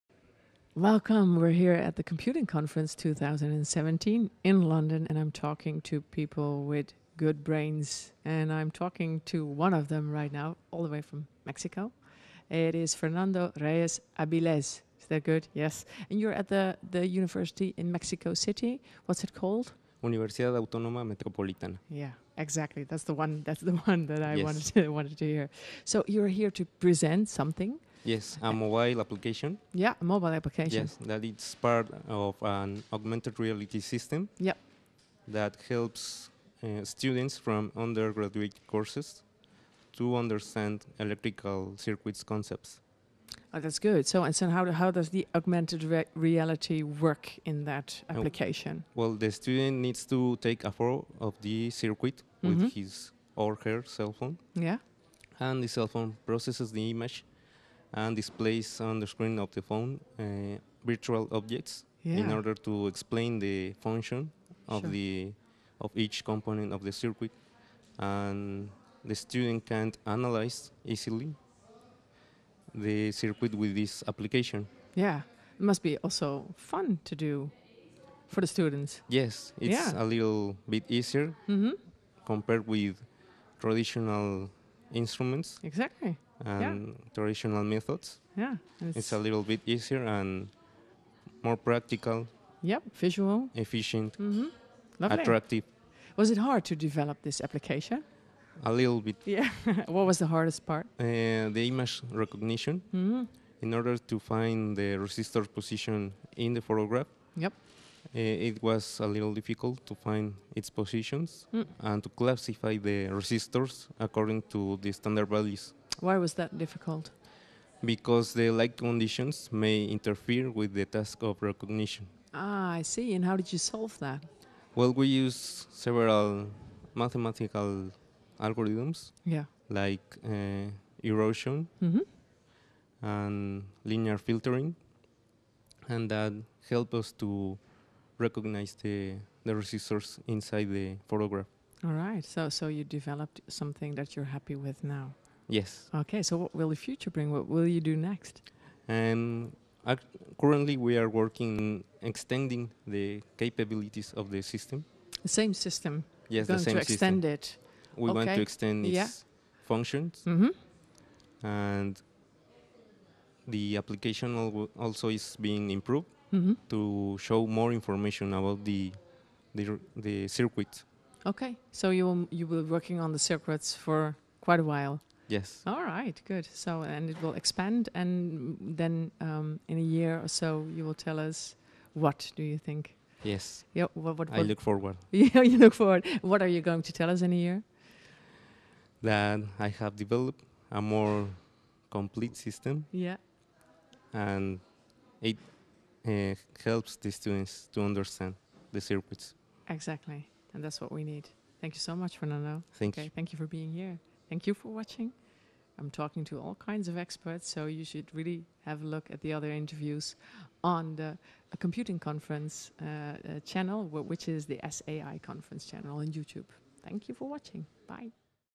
Interview with Fernando Reyes Aviles from Universidad Aut´onoma Metropolitana, Mexico
Fernando Reyes Avil´es is a Master’s Degree in Computer Science student at Universidad Aut´onoma
Metropolitana, Mexico City, Mexico. He studied a Bachelor of Science in Electronic Engineering
in the same university. He also worked there as a Teaching Assistant for three years. Currently, He
is finishing his Master’s Degree and He is presenting the core contribution of his thesis project at
Computing Conference 2017.

His current research is focused on the improvement of the augmented reality system He developed
during his Master’s Degree thesis project. He is incrementing the capabilities of both,
the measurement instrument and the mobile application (i.e., virtual objects generation and scene
recognition algorithm), In order to enlarge its possible applications.

Interviewer: Kim Coppes

Abstract:
This paper presents an implementation of a mobile augmented reality system, which was designed to help the understanding of resistive electric circuits. The system employs a smartphone, a measuring instrument, and an electric circuit built on a breadboard. The smartphone captures a photograph of the circuit and receives data from the measuring instrument, which it uses to generate a layer of augmented reality information that is merged with the original image and displayed on the screen of the smartphone. In order to display augmented reality on a real circuit, it was developed a markerless method to identify resistors from a photograph of the circuit and set virtual objects over it. The results showed that the AR system is stable enough to be considered as a good first step in the development of a markerless AR system which works on real resistive circuits.